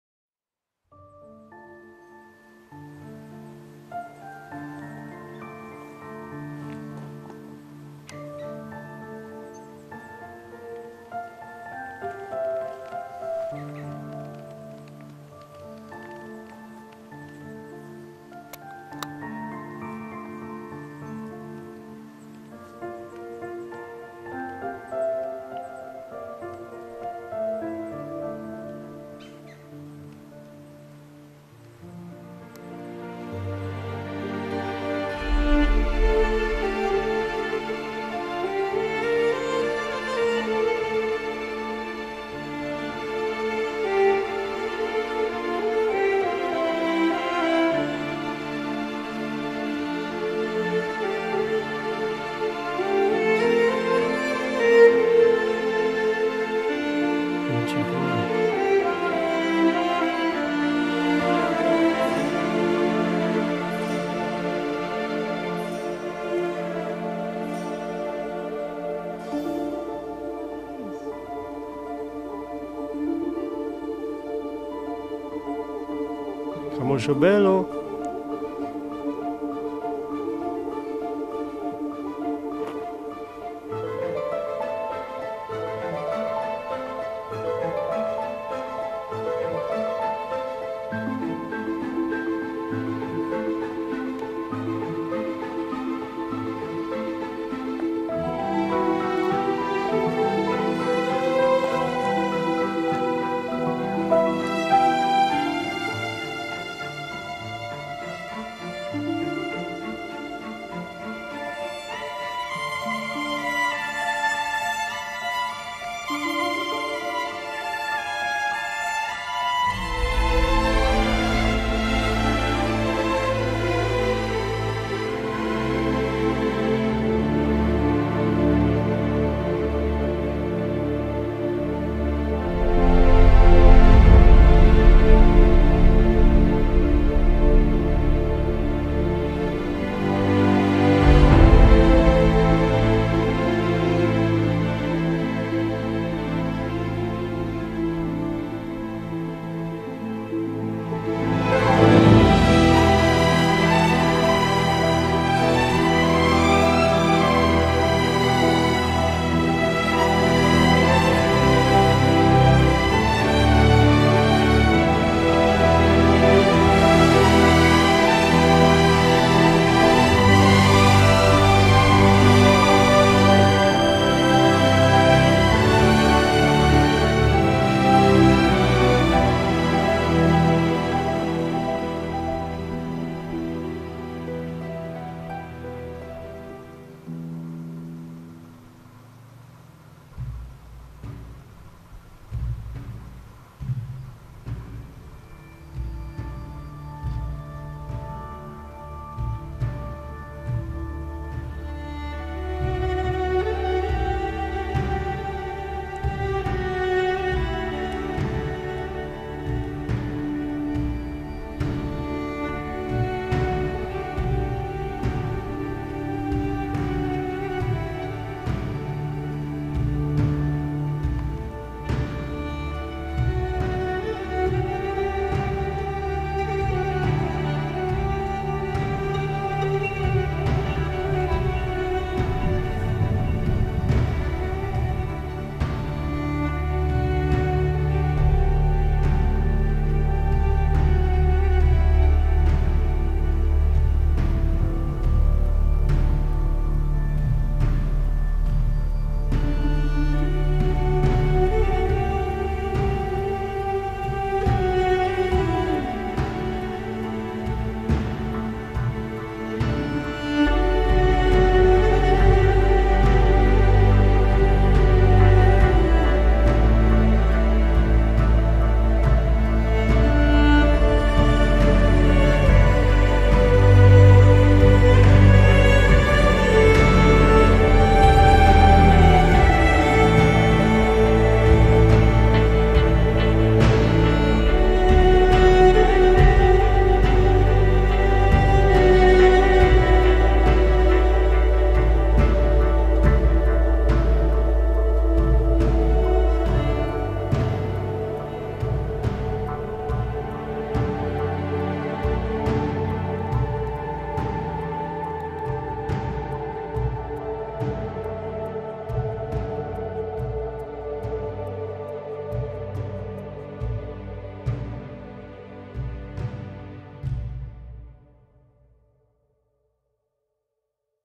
PNALM: Amore in estate(camosci del Monte Amaro)
Salendo su per la montagna vidi il camoscio, l'animale bello e puro. Il fantastico animale mi mostrò la sua anima, nei suoi occhi vidi l'innocenza, nel suo passo la sua bellezza... avrei voluto abbracciarlo, fargli sentire il mio amore, ma lui era indifferente, forse lo già sapeva.